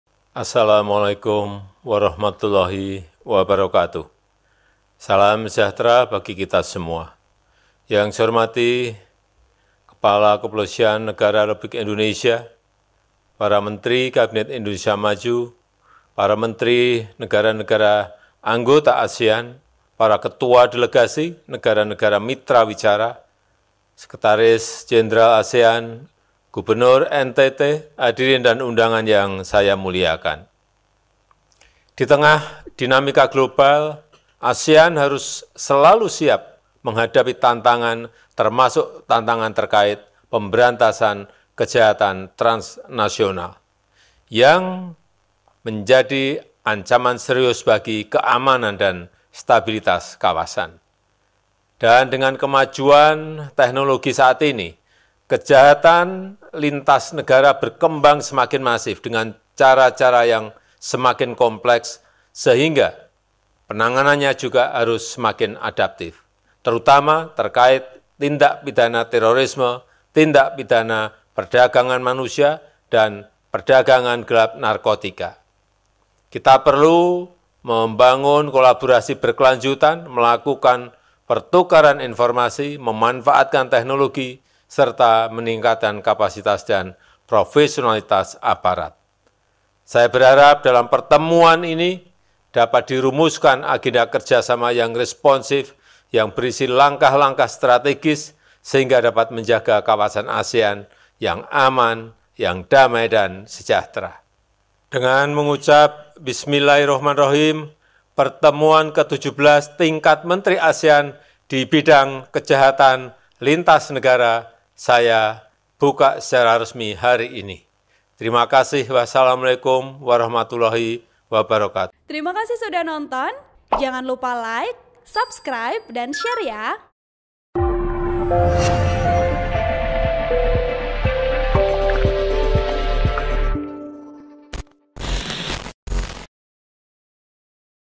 [FULL] Pidato Presiden Jokowi Buka ASEAN AMMTC ke-17, Bahas Kejahatan Transaksional hingga Teroris
TRIBUN-VIDEO.COM - Presiden Jokowi membuka ASEAN Ministerial Meeting on Transnational Crime (AMMTC) ke-17 di Labuan Bajo, Manggarai Barat, Nusa Tenggara Timur (NTT), Senin (21/08/23) pagi.

Dalam pidatonya, Presiden Jokowi membahas soal penanganan kejahatan transaksional di negara ASEAN.

Termasuk membahas penanganan terorisme di ASEAN.

(Tribun-Video.com/RS)